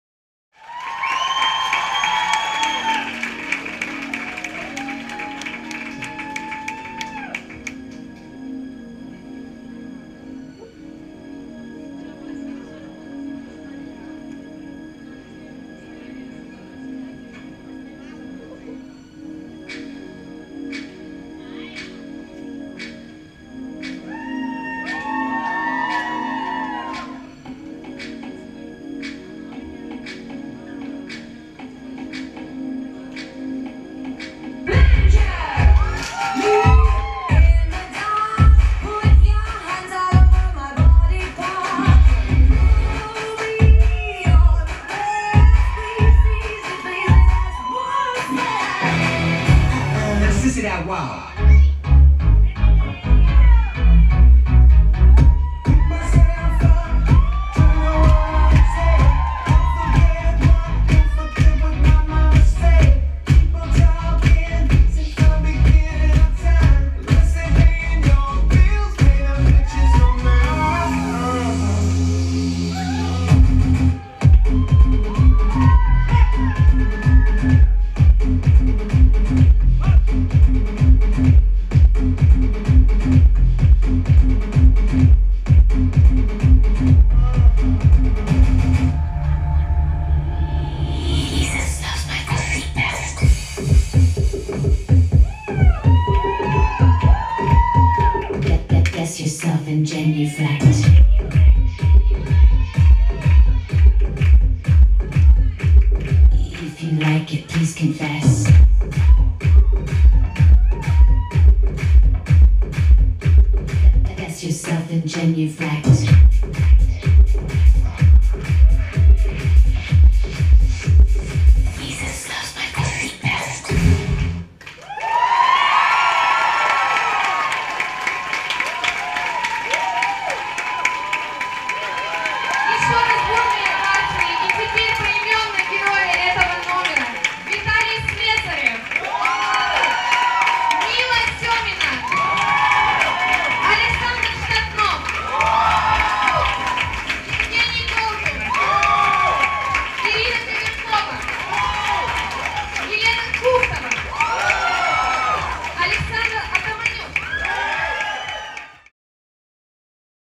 Hard Candy Fitness x Planet Fitness All Stars Dance Team @ PFDC
Performance: Hard Candy Fitness x Planet Fitness All Stars Dance Team
Video: Evgeny Shumikhin
Camera: GoPro HERO3+
Location: Planet Fitness Dance Competition